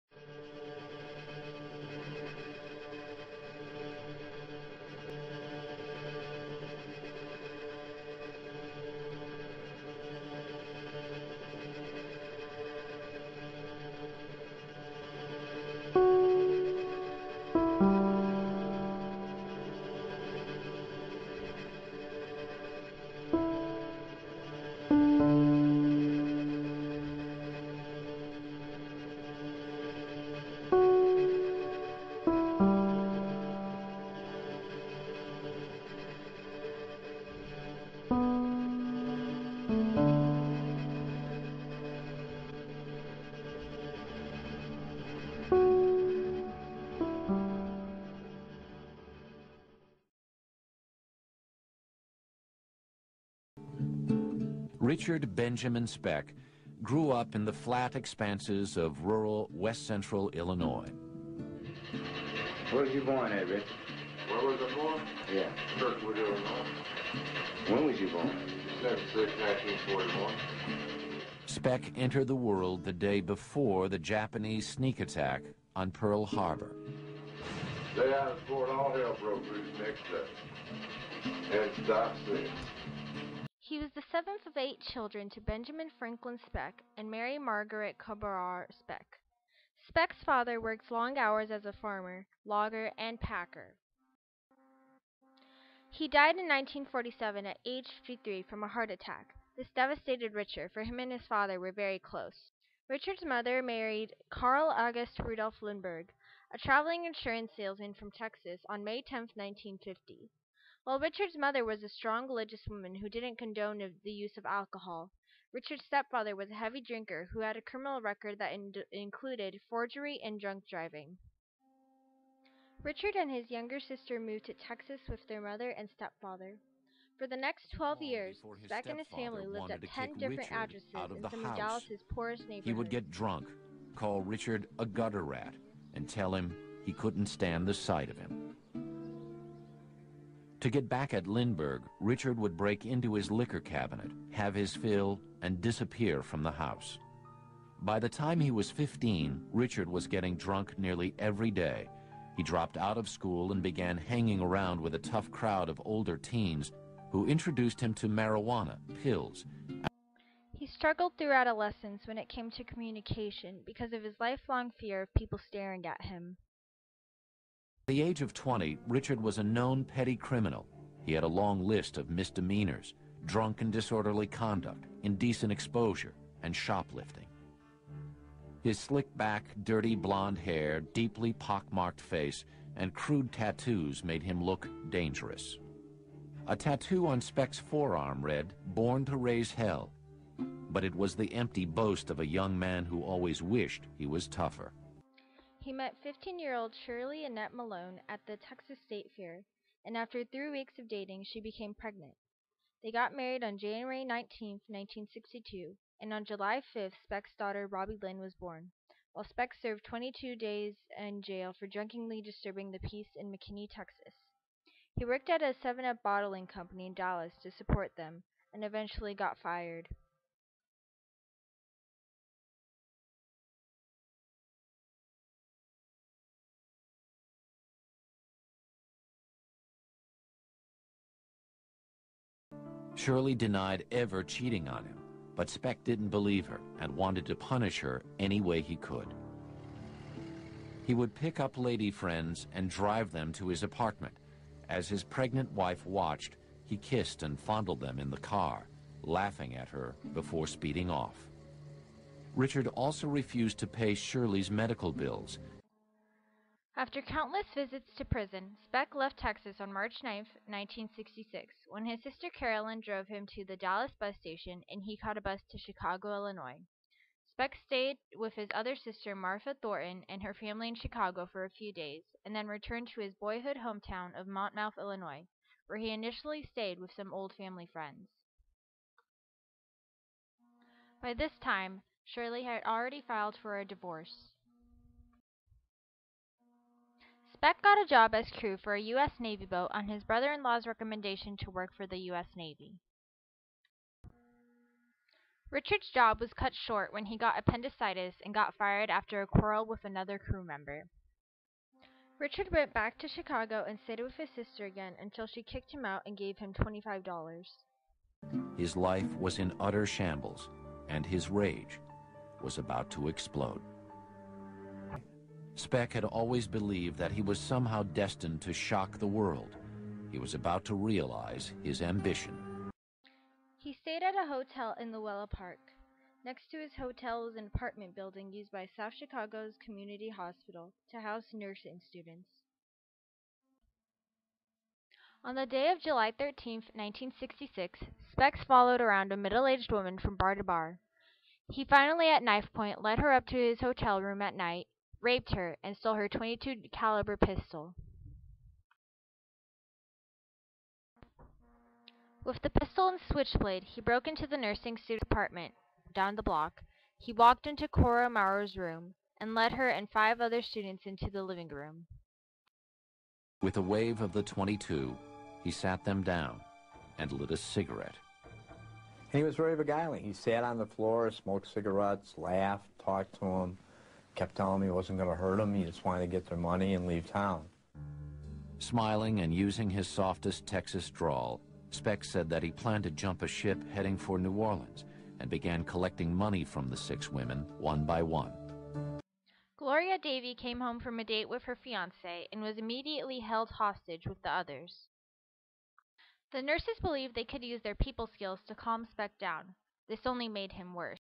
Mariett Romtvedt - Richard Speck Part 1
Richard Speck's life leading up to his famous murders.